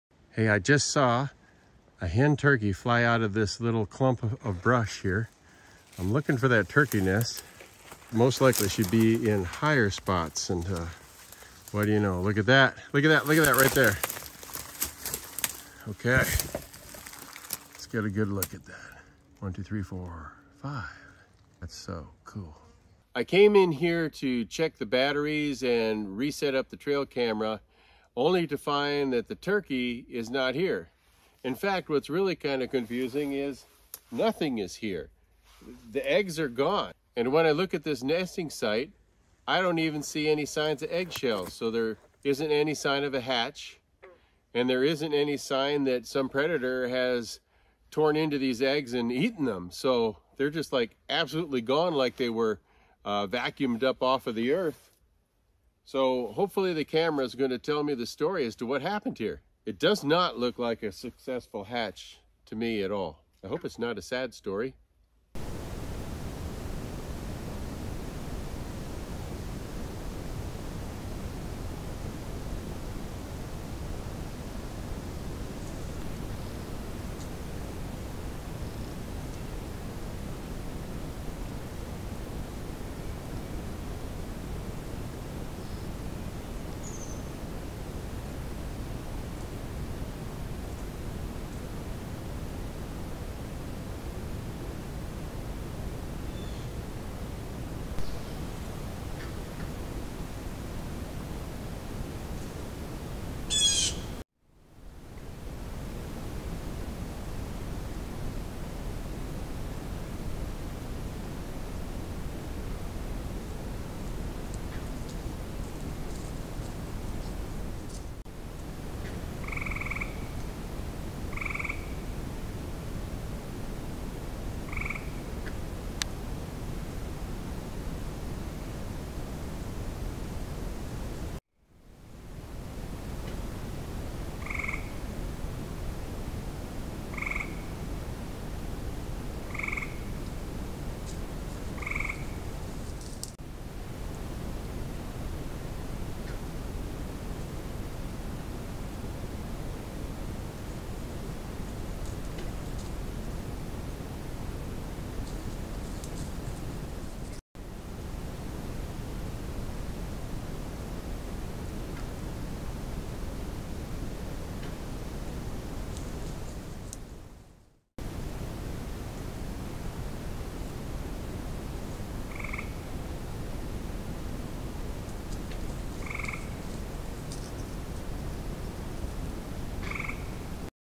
Solving the Turkey Nest Mystery???
I was excited to catch sight of a wild turkey hen running in the woods and then found her nest.  I put a camera on the spot to see if she would return and hatch them...